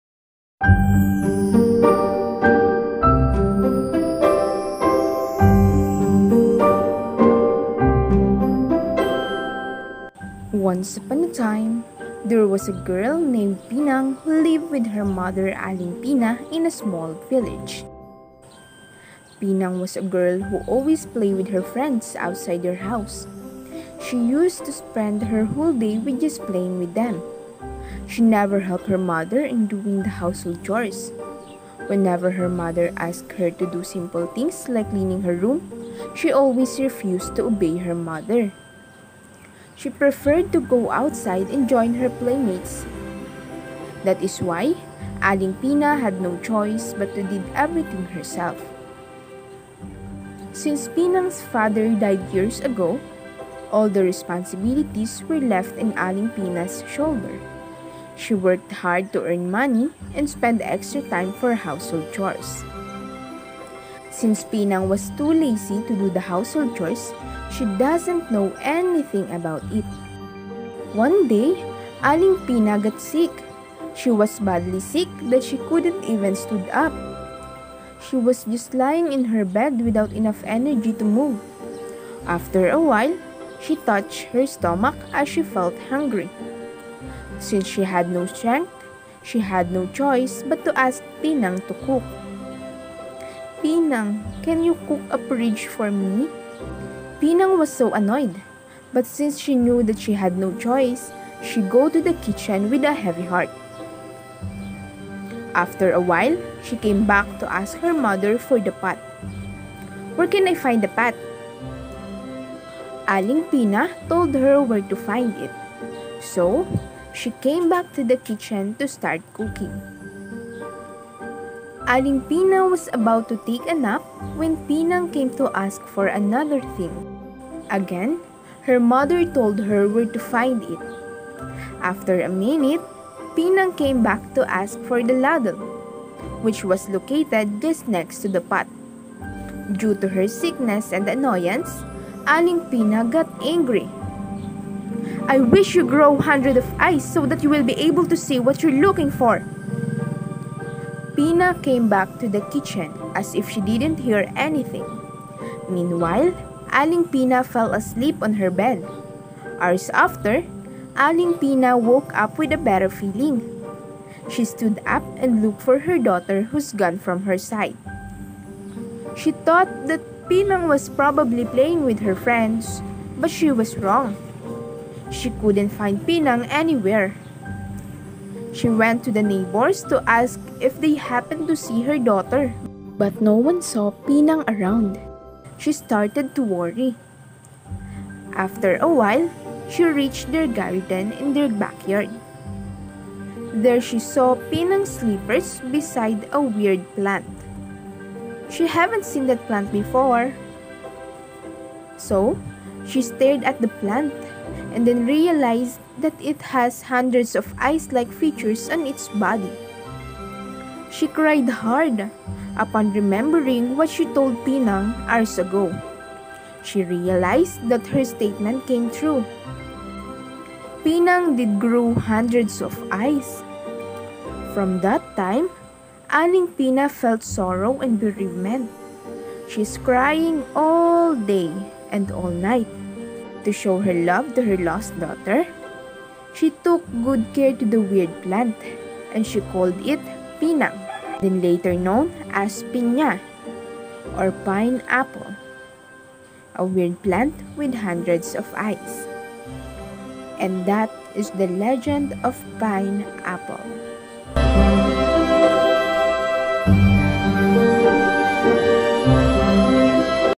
THE LEGEND OF PINEAPPLE (ANG ALAMAT NG PINYA)
This story is all about a girl named Pinang who became a weird plant called Pineapple.

Background Music
Magical Fairy Tale Background Music For Videos/Medieval Fantasy Music/Atmospheric Ambience